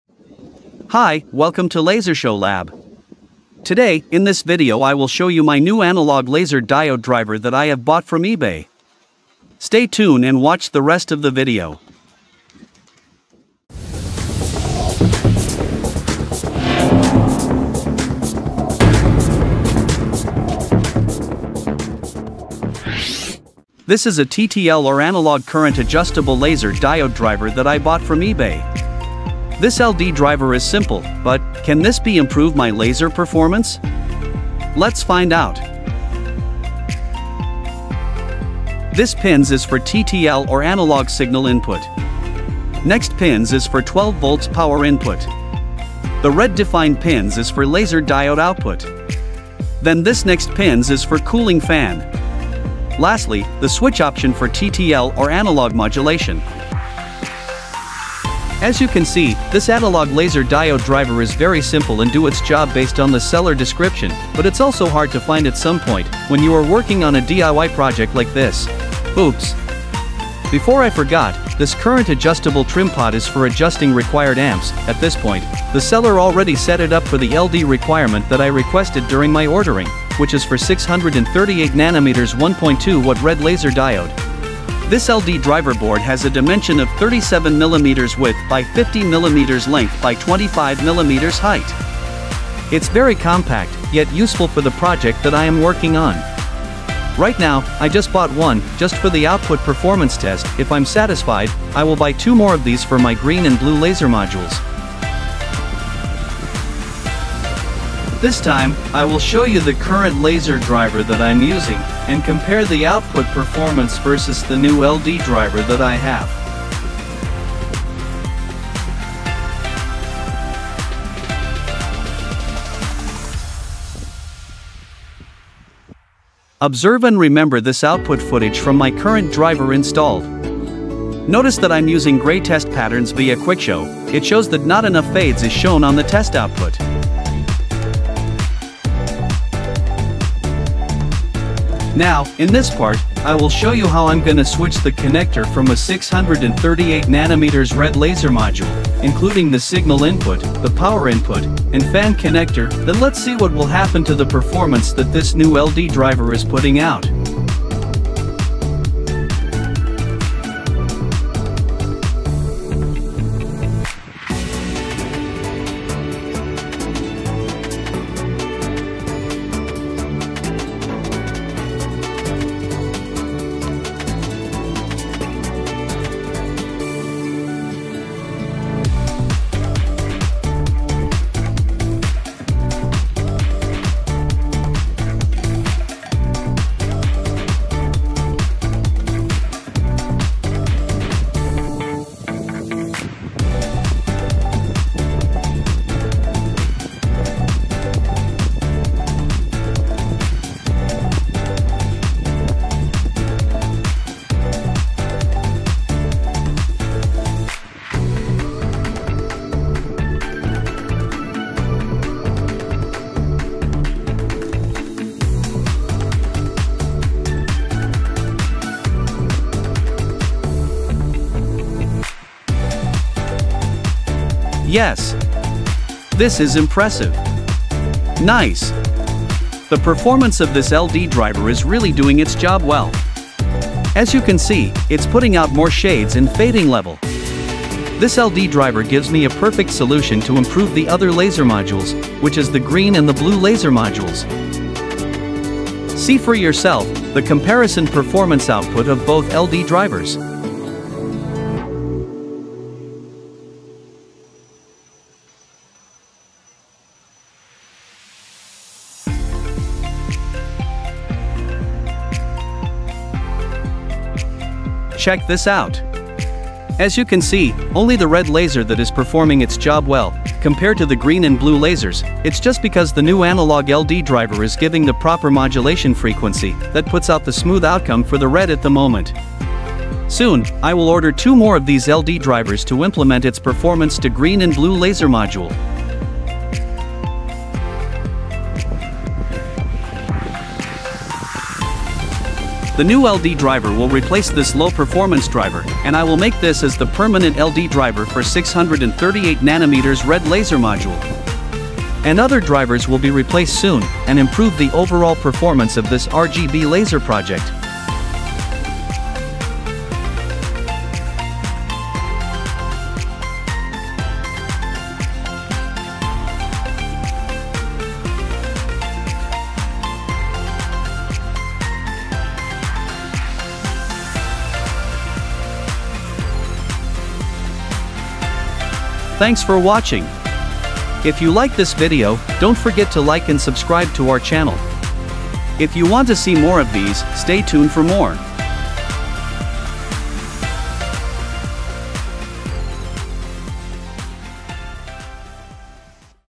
Laser Diode Driver Test & Comparison: Which One Performs Best?
In this video i will show you my new analog laser diode driver that i have bought from ebay. Stay tune and watch the rest of the video.